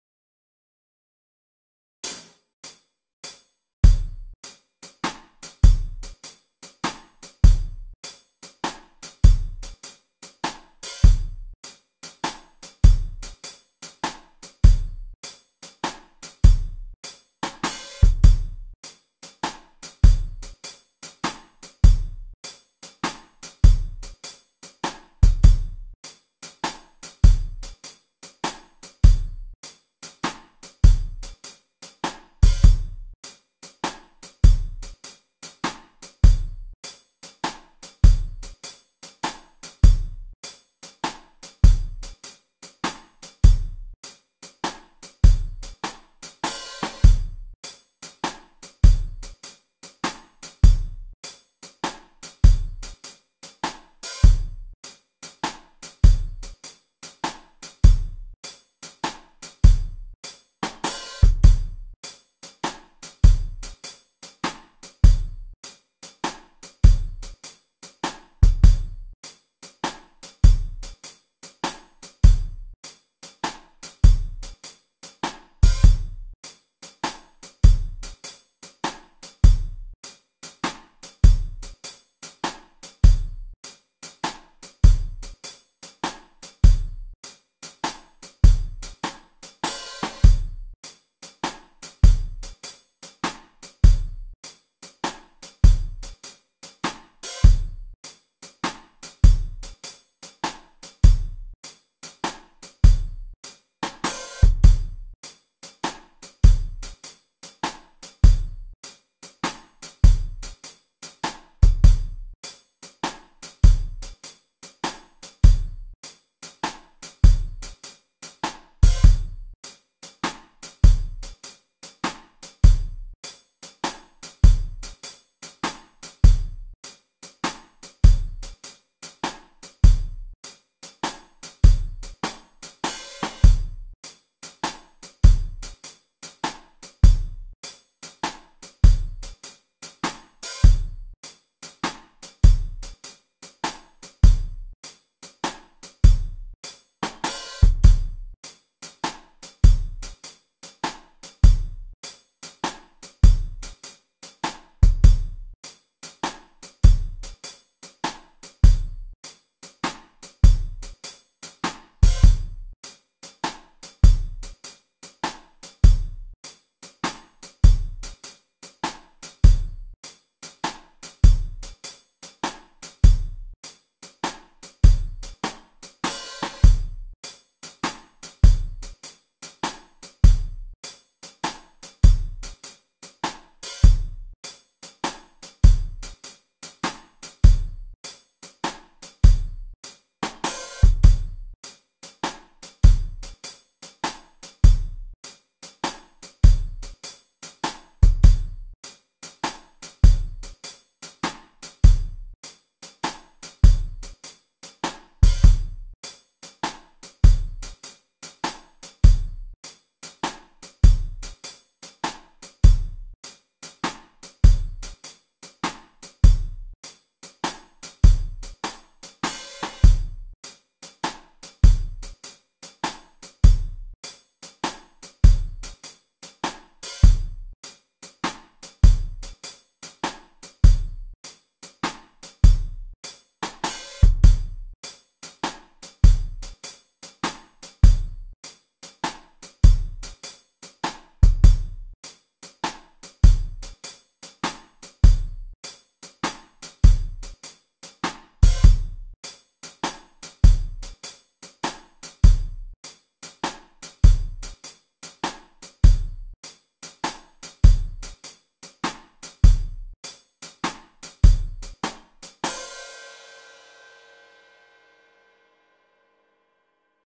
Country Waltz Drum Track (100 BPM - 3/4 Swing Feel)
A drums only backing track / drum track / drum loop for practicing, jamming and improvising.

At the bottom left you can see a metronome, at the top right the song structure with the indication where you are in the song.

Title: Country Waltz Drum Track
Time signature: 3/4 swing feel
Tempo: 100 beats per minute
Song structure: 8 bars